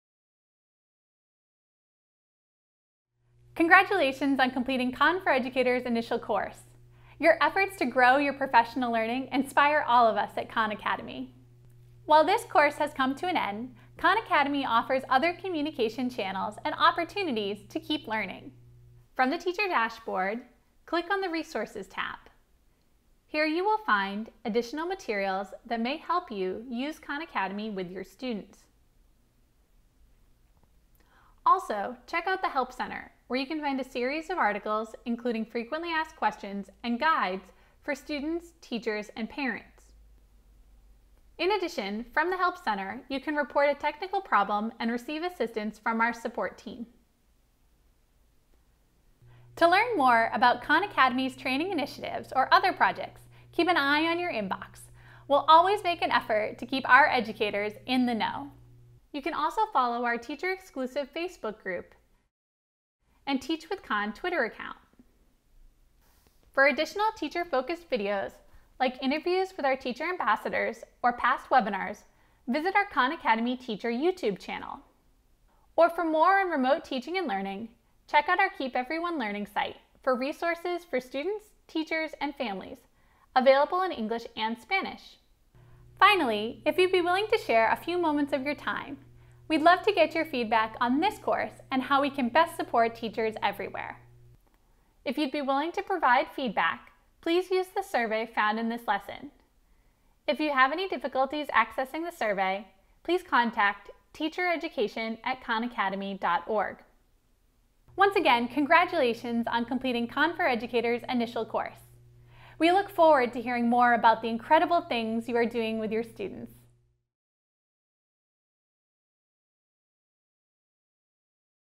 Khan for Educators: Where do I go from here?
Where do I go from here? is part of Khan for Educators, a free course for teachers to learn more about Khan Academy, the content and tools available for teachers and students, and best practices for implementing technology with students.